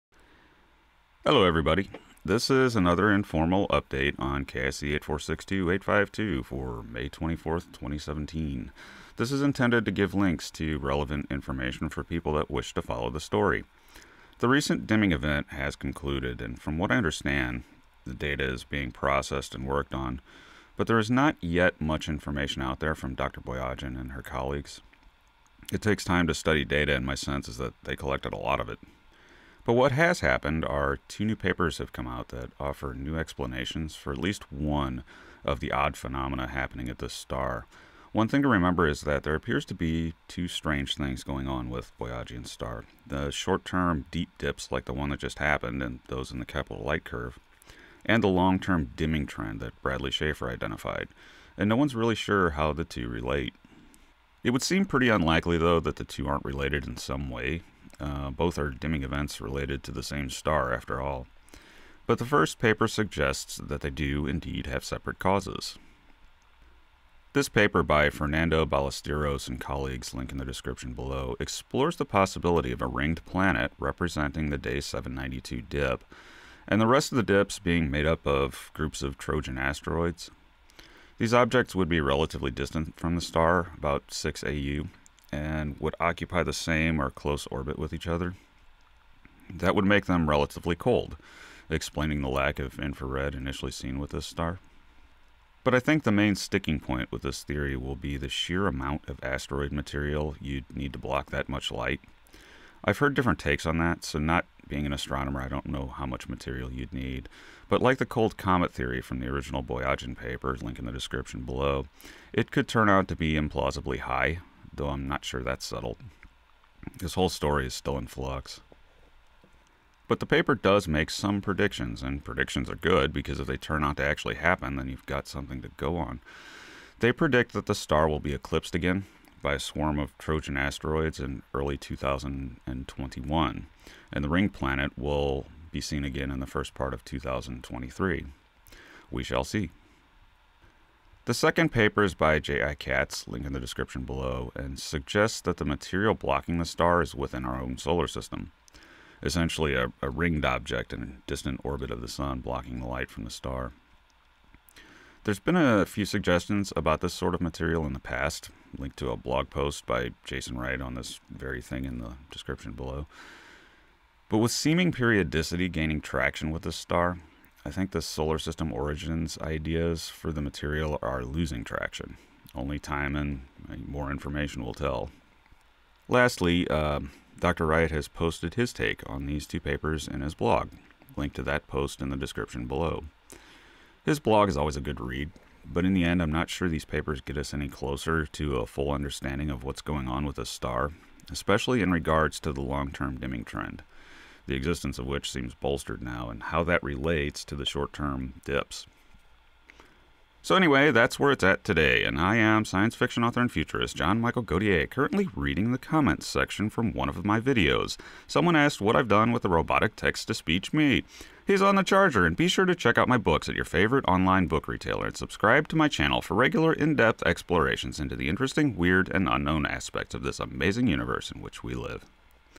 KIC 8462852 Update for 05/24/2017
An update in my continuing coverage of KIC 8462852 or Boyajian's star, or the WTF star, or Tabby's star for May 24, 2017.

Resources and Links:

"KIC 8462852: Will the Trojans return in 2021?", F. Ballesteros et al., 2017

"Planet Hunters X. KIC 8462852 - Where's the Flux?", T.S. Boyajian et al, 2015

"Tabetha's Rings", J.I. Katz, 2017